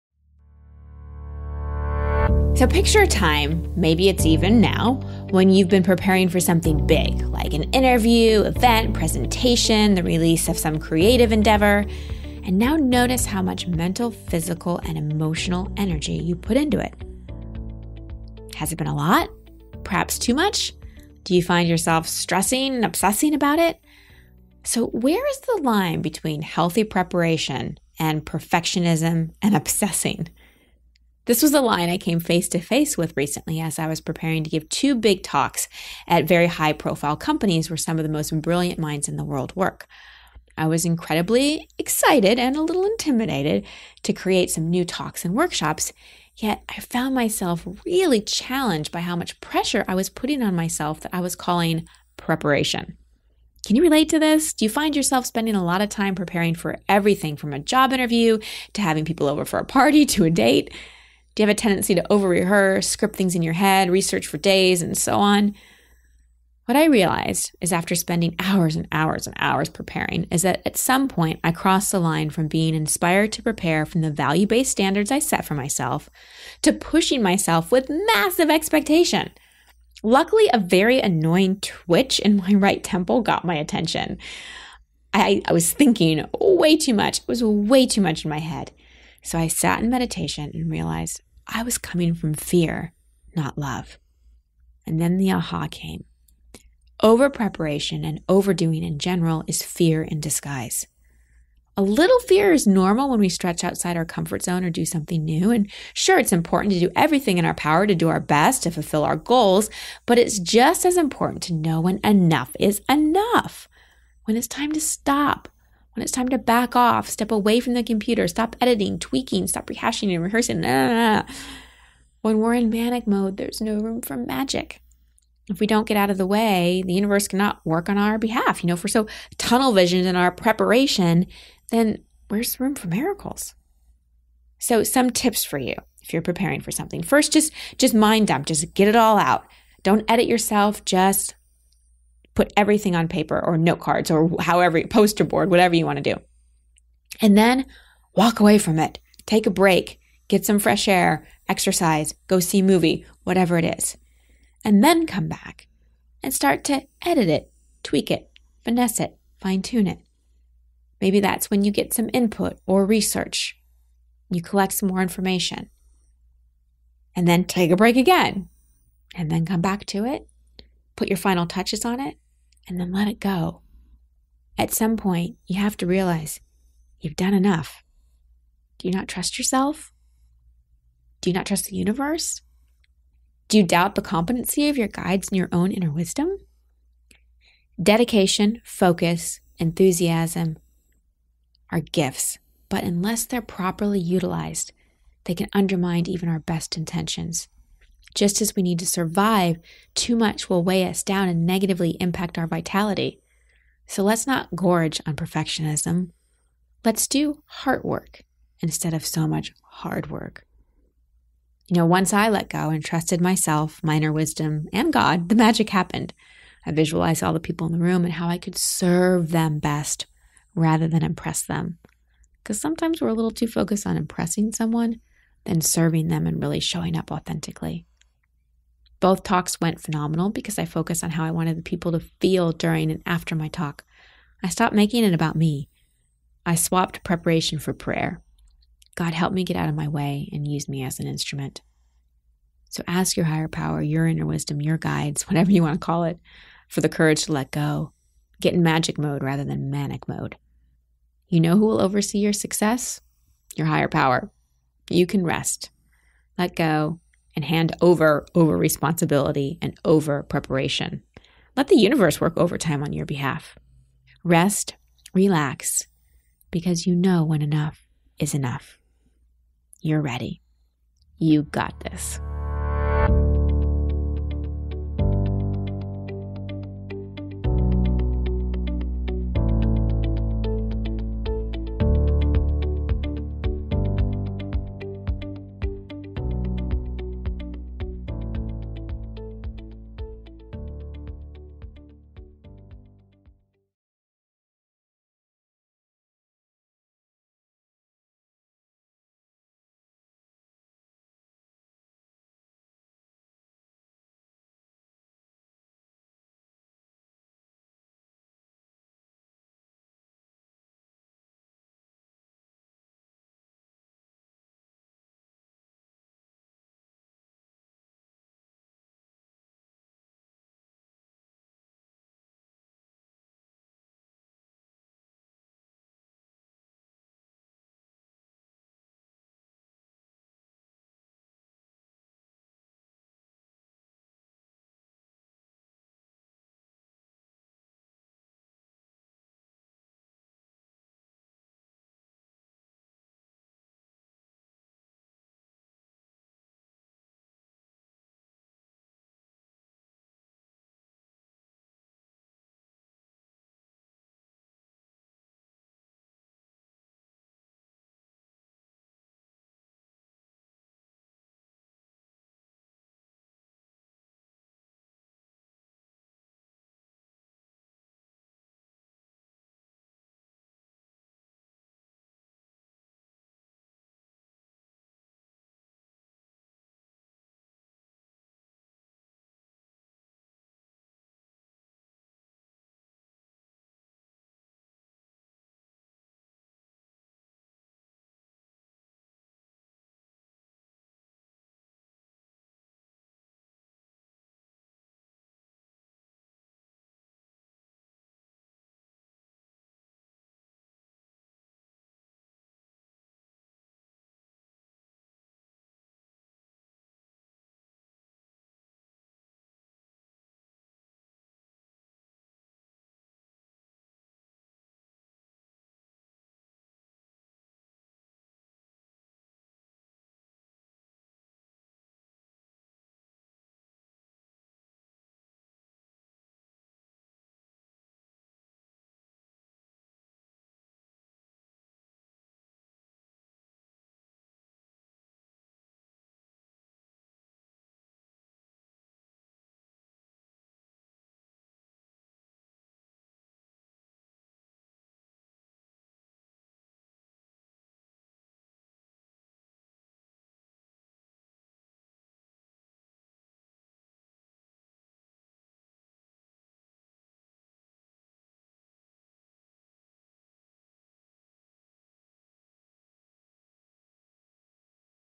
CC: Where is the line between healthy preparation and perfectionism/obsessing? This is the question
Where's the line between preparation and obsession?
Picture a time, maybe it is even now, when you have been preparing for something BIG like an interview, event, presentation, or the release of some creative endeavor. Now consider how much mental, physical, and emotional energy you put into it.
Was it a lot -perhaps too much? Did you find yourself stressing out about it?